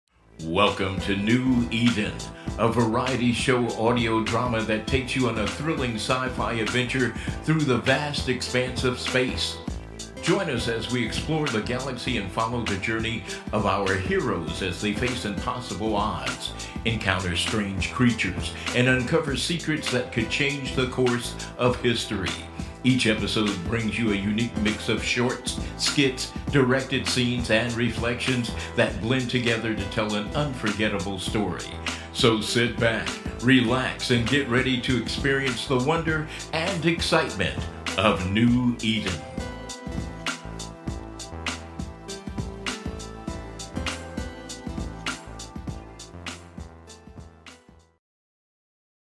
Teaser - New Eden Podcast Intro (made with Spreaker)
Since our podcast is an audio drama variety show format, I thought it fitting to go with an old Radio-Esque format combined with futuristic settings, and themes. The end result being a retro-futuristic cyberpunk mashup. I hope you enjoy!

I would like to say a special Thanks for my Dad for recording the intro to this. My dad has been wanting to use his vocal talents for while, and I am happy that I could provide him with an opportunity to flex his skills acquired in his radio days.